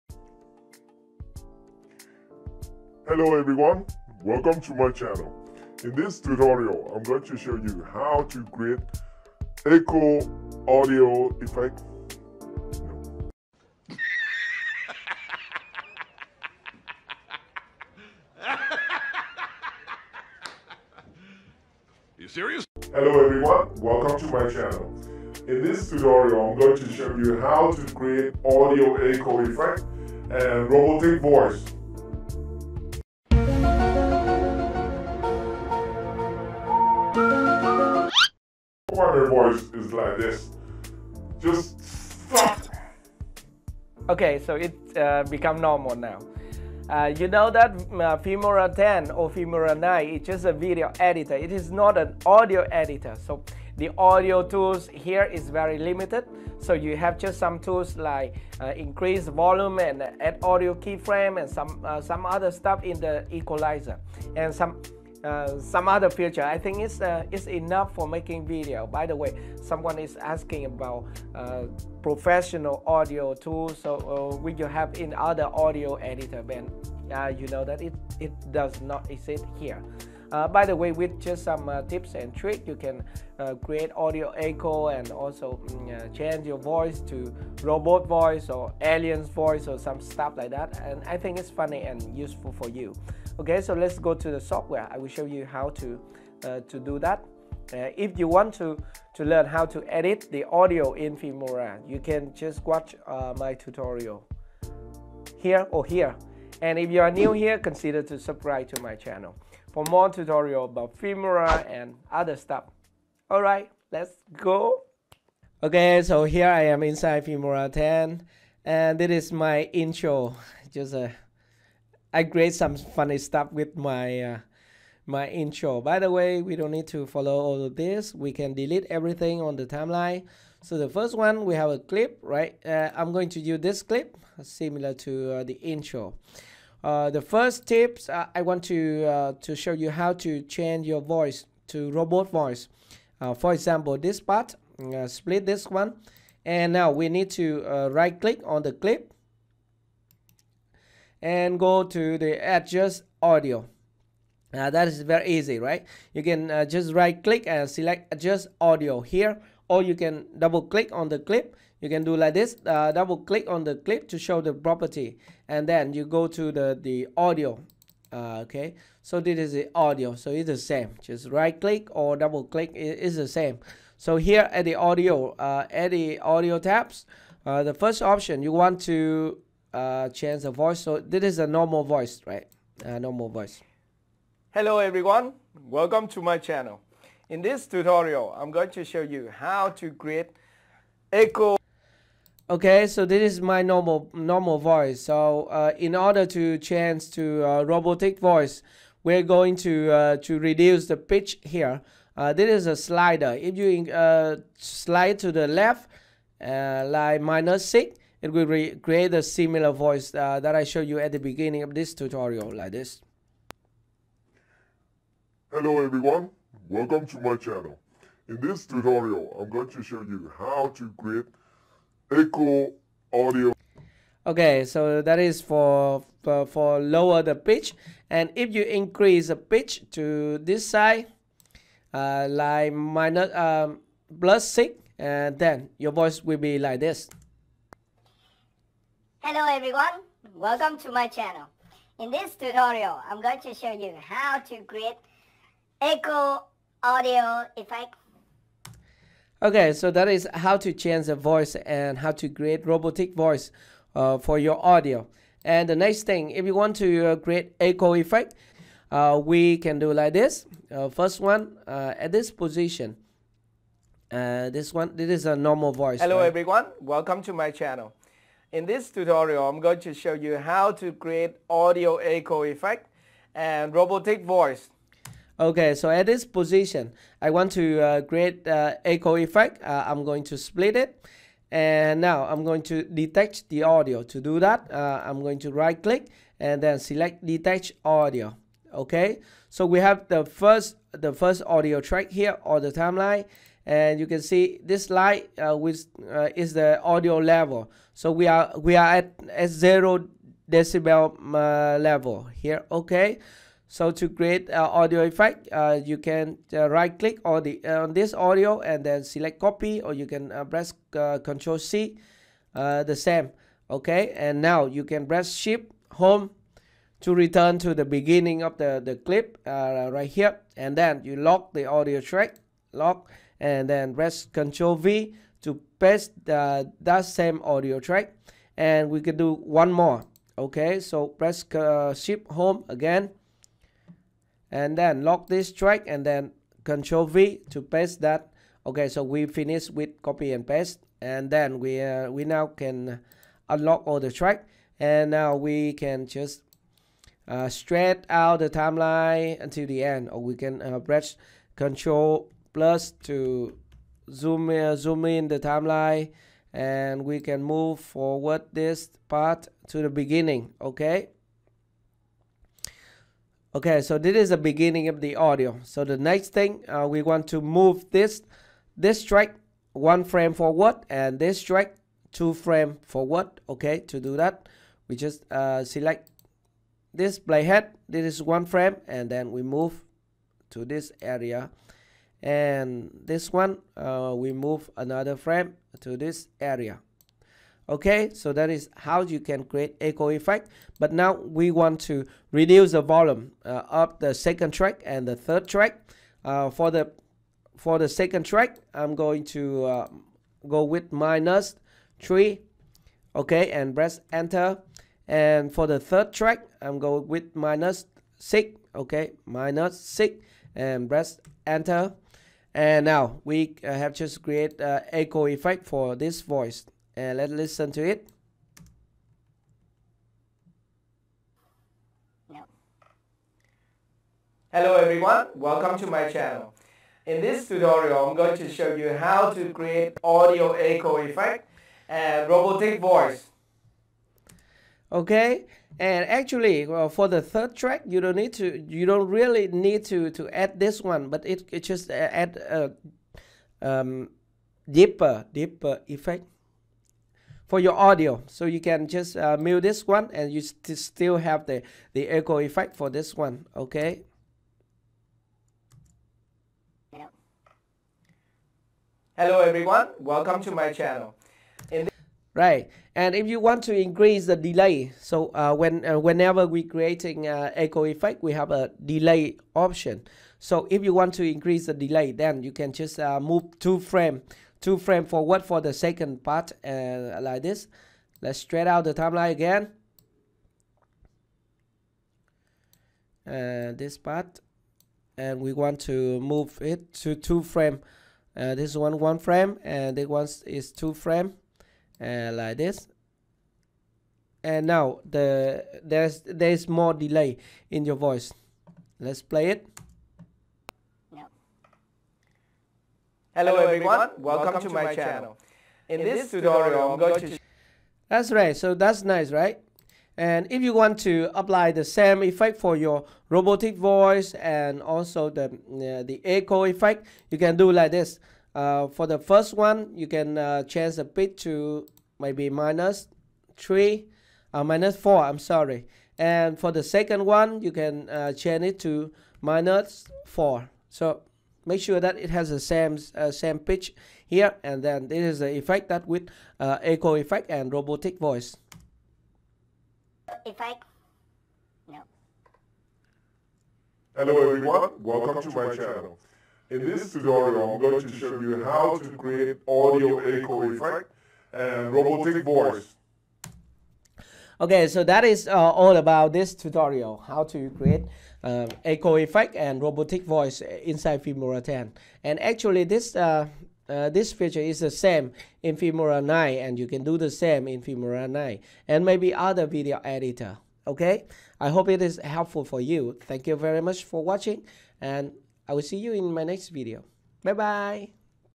How to Create Audio Echo Effect and Robotic Voice in Filmora Video Editor
This tutorial shows you how to create audio echo effect in Filmora as well as how to create robotic voice using Filmora video editor.